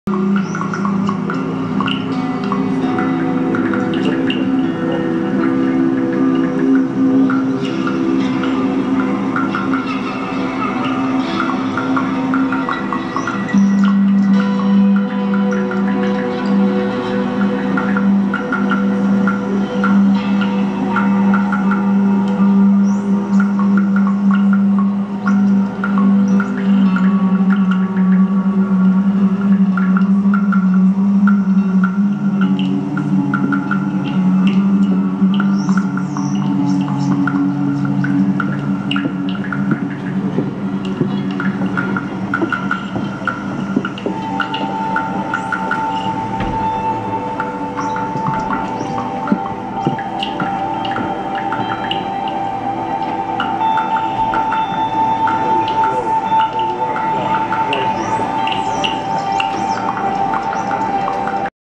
"Butoh & Noise art" Shiro＋Teruyuki Kurihara Live performance pt.1
"Butoh & Noise art"
Shiro(Butoh)＋Teruyuki Kurihara(Noise) at Wabi Sabi Garden, St kilda, Melbourne, Australia 22/06/2013